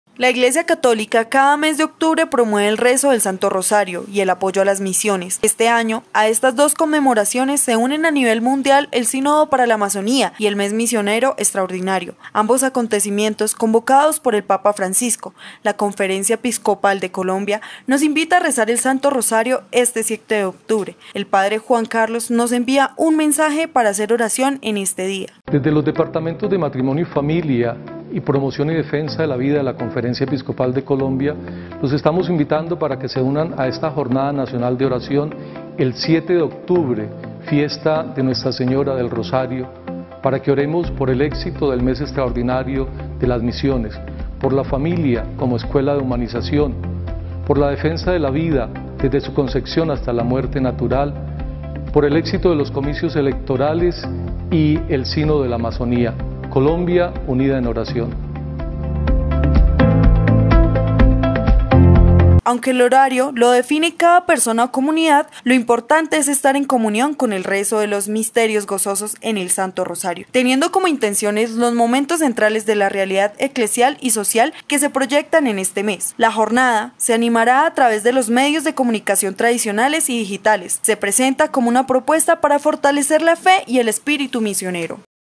7 de octubre, Jornada nacional de oración
Este 7 de octubre se celebra la jornada nacional de oración, el padre Juan Carlos a través de la conferencia episcopal colombiana, envía un mensaje que nos invita a rezar por los momentos centrales de la realidad eclesial y social que se proyectan para este mes.